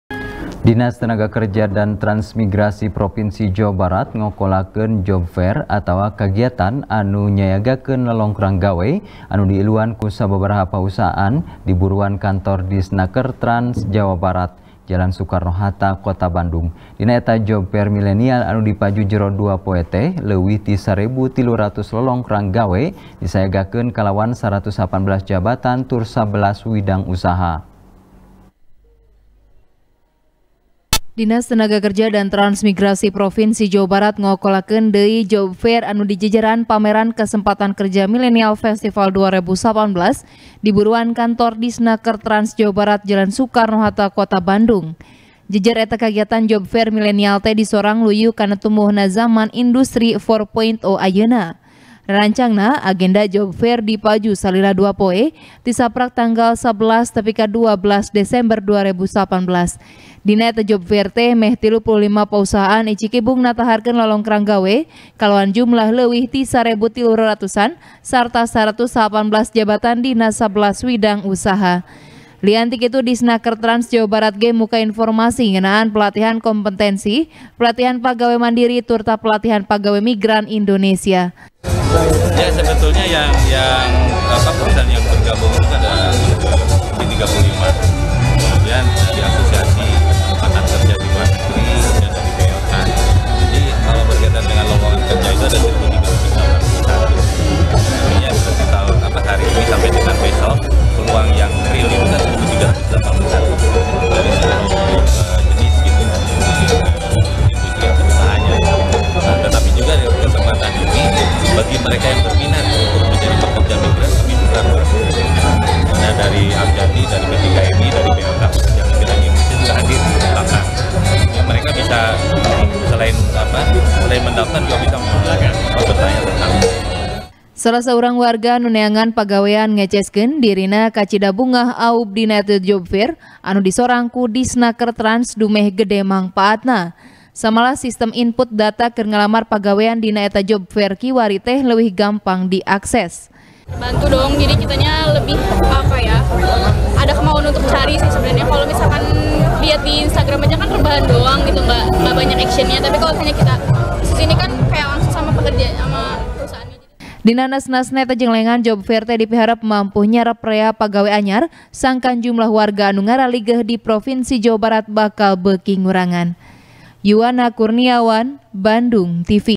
DISNAKERTRANS JABAR Nyorang Job Fair Milenial 2019 | TP | BANDUNGTV
TP | BANDUNGTV | IWAN M RIDWAN |Dinas Tenaga Kerja Dan Transmigrasi Provinsi Jawa Barat, Ngokolakeun Job Fair Atawa Kagiatan Anu Nyayagakaeun Lolongkrang Gawe, Anu Diiluan Ku Sababaraha Pausahaan, Di Buruan Kantor Disnakertrans Jawa Barat, Jalan Sukarno Hatta, Kota Bandung. Dina Eta Job Fair Milenial Anu Dipaju Jero Dua Poe Teh, Leuwih Ti Sarebu Tilu Ratusan Lolongkrang Gawe Disayagakeun Kalawan 119 Jabatan Tur Sabelas Widang Usaha.